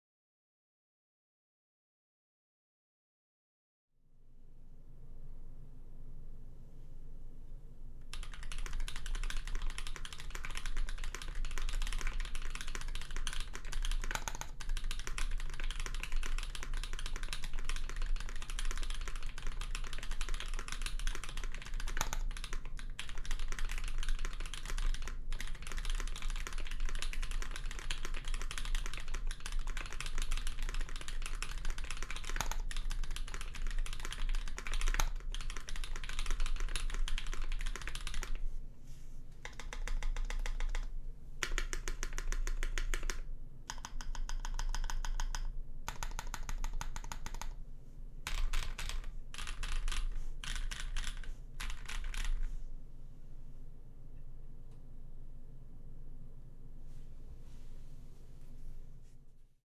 Alpine65 with Lubed Creamsicles Sound Test
Keyboard: Bitmap Studios Alpine65 in E-White
Plate: FR4
Keycaps: GMK Umbra
Switches: Creamsicles (Cream stem, Tangerine V2 housing) lubed with 205g0, TX films
Springs: TX Long lubed with 105
Sound dampening: None
Stabilizers: C3 lubed with 205g0